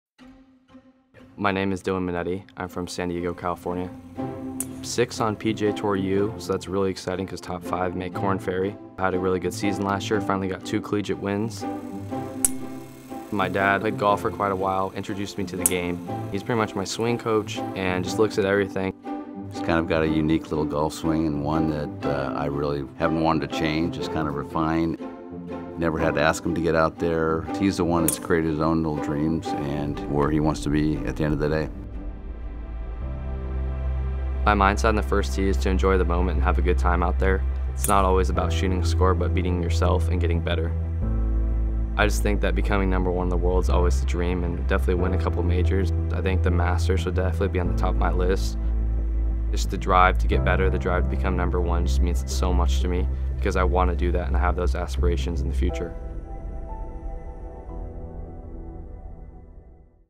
Meet Dylan Menante | Inside the Elite Amateur Golf Series
On a journey to be the best. Learn more about Dylan Menante's journey to defend his title at the 2022 Northeast Amateur and etch his name into the rich history of American amateur golf.. and beyond.